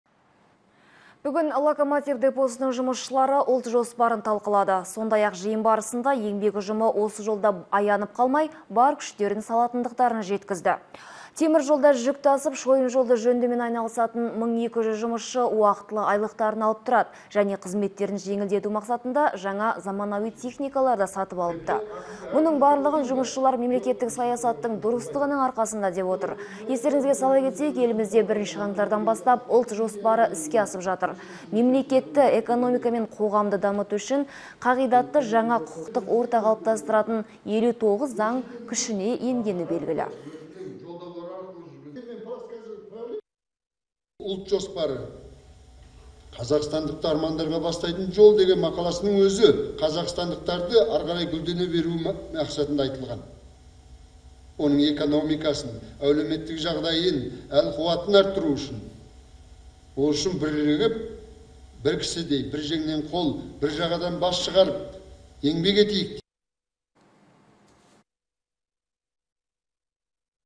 Алматы локомотив депосының жұмысшылары Ұлт жоспарын талқылады (17.02.16)
Жиын барысында еңбек ұжымы осы жолда аянып қалмай, бар күштерін салатындықтарын жеткізді.